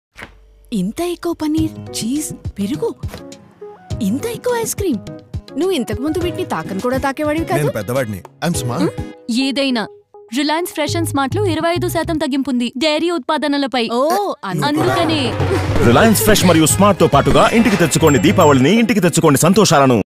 Offer on Dairy | TEL | Reliance SMART
Iss Diwali dahi, Milkshake aur Paneer hoga unlimited, bilkul Diwali mein khushiyon ki tarah. GharLaoDiwali, Ghar Lao Khushiyaan!